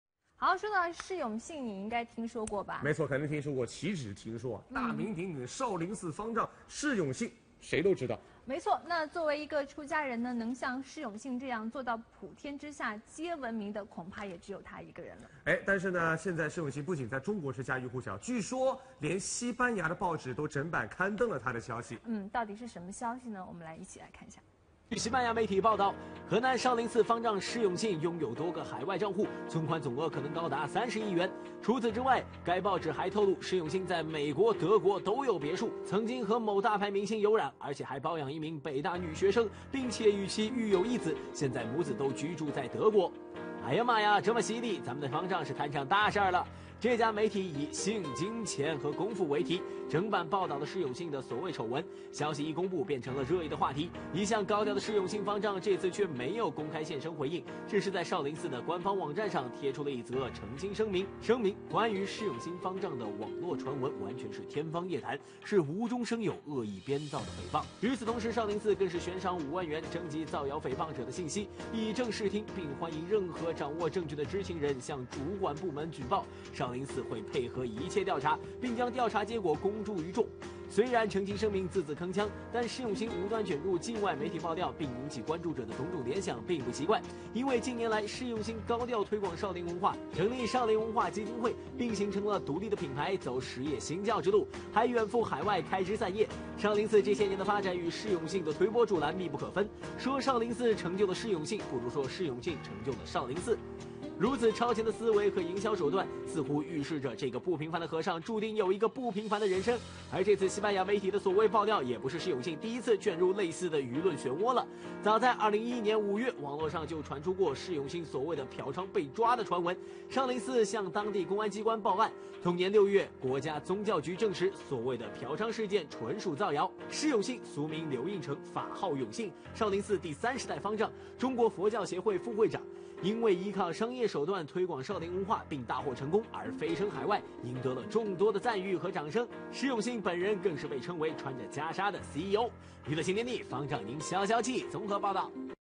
少林寺回应方丈私生子传闻：旧闻重炒 无稽之谈
近日网上有传言，称西班牙媒体爆料，河南少林寺方丈释永信有北京大学生情人，私生子在德国以及有巨额海外存款。15日，嵩山少林寺方丈室对此表示，这些都是两年前的谣言重炒，纯属胡言乱语。